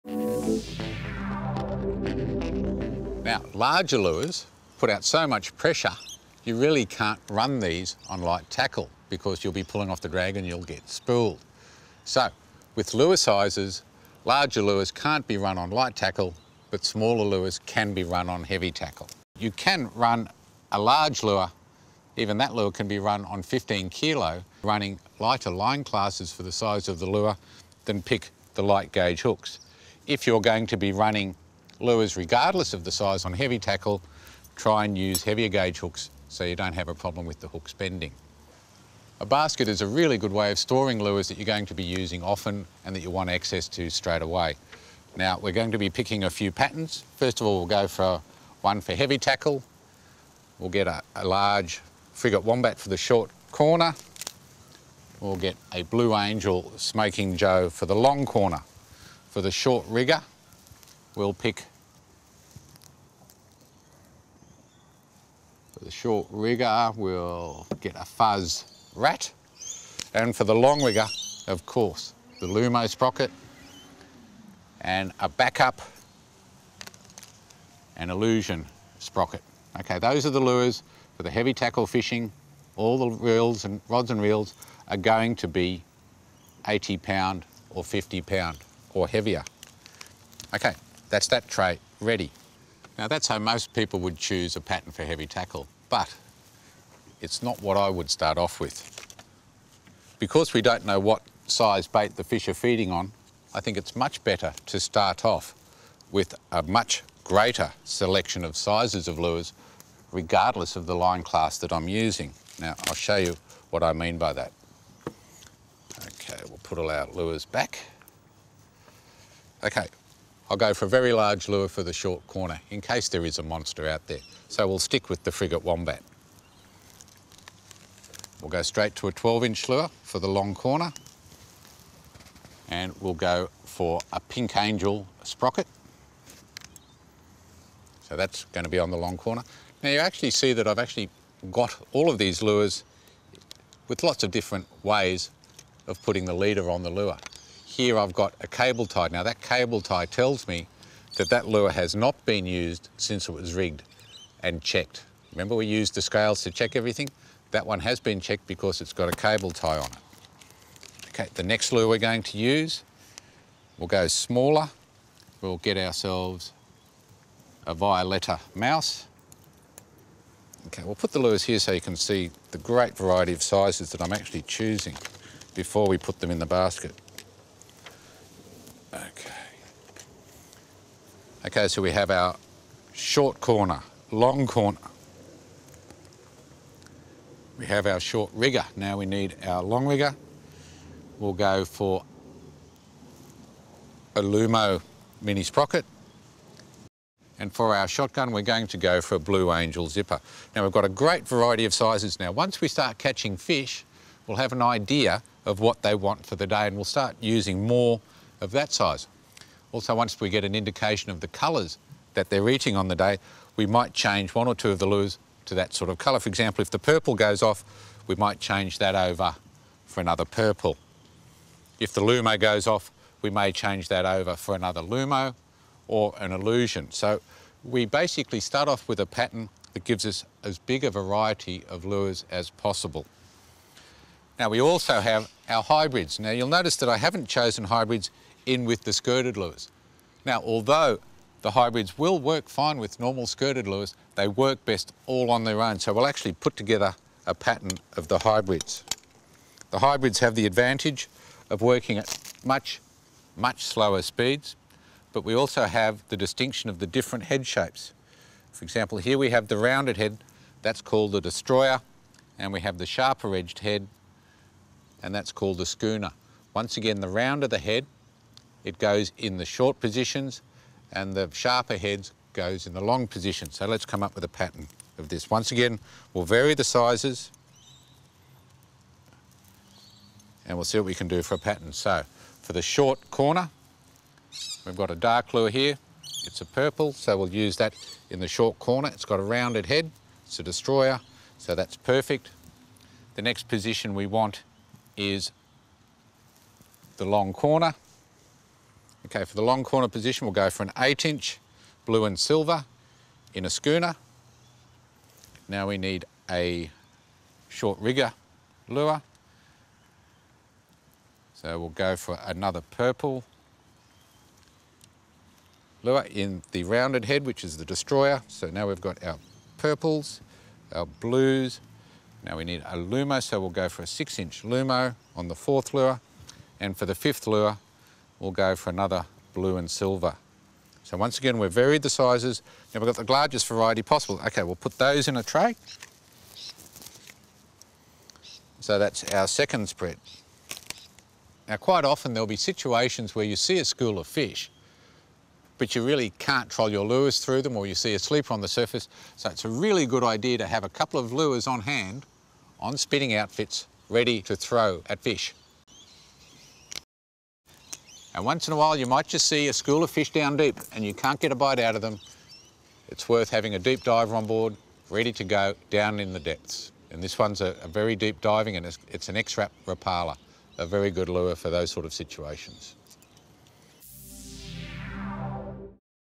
Lure Choice Part 2.mov
Peter Pakula explains how to put a trolling lure pattern togrether. Part 2 of 2